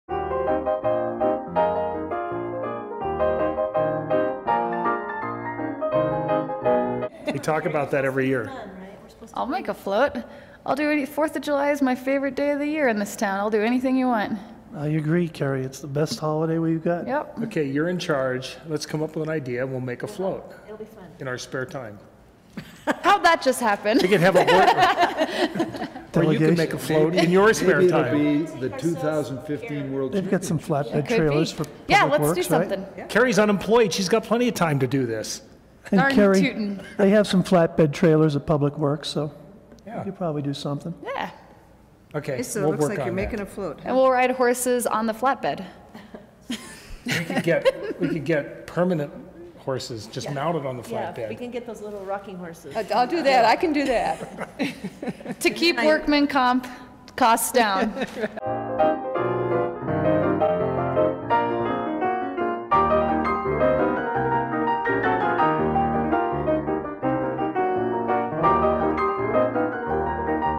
Kerry Donovan wants to be a Senator...
Kerry Donovan is running for state Senate in Colorado. We checked out some footage from her time on the Vail Town Council...